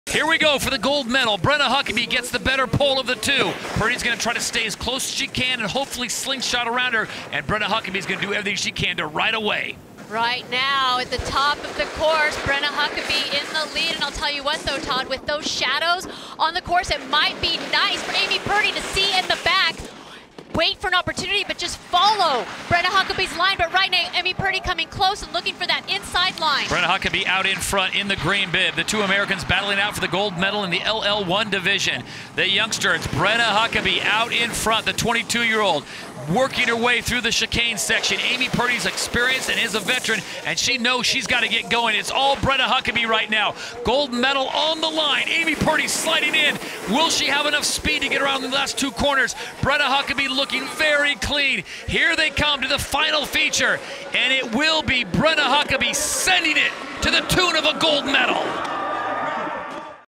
Team USA 2018 Playlist: Brenna Huckaby Takes Home Gold
Brenna Huckaby battles with veteran Amy Purdy to win gold at the Olympic Winter Games PyeongChang 2018.